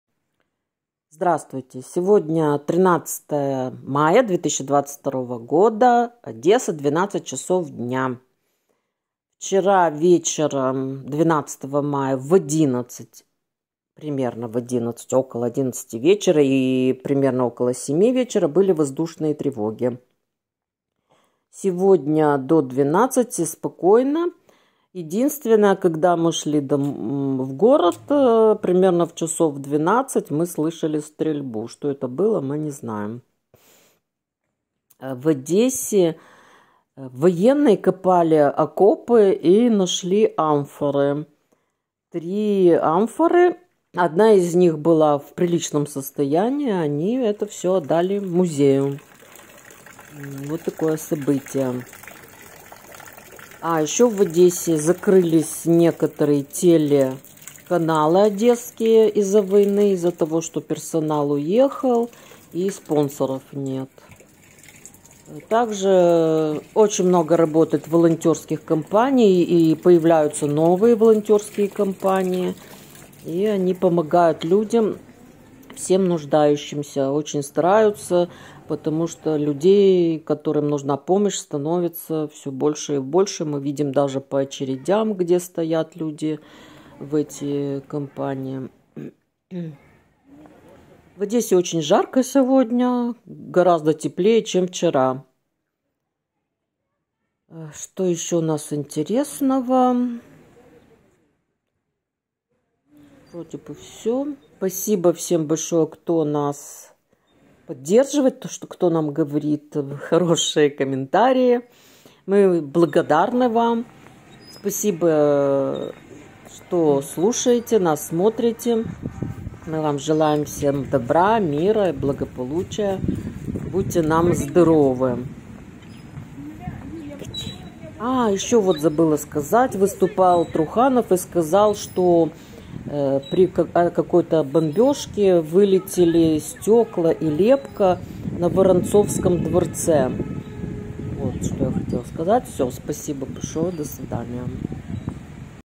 Одесса. 13 мая 2022.
В городе наступило настоящее лето. +30, на солнце.

Видео Ирина Синюкова.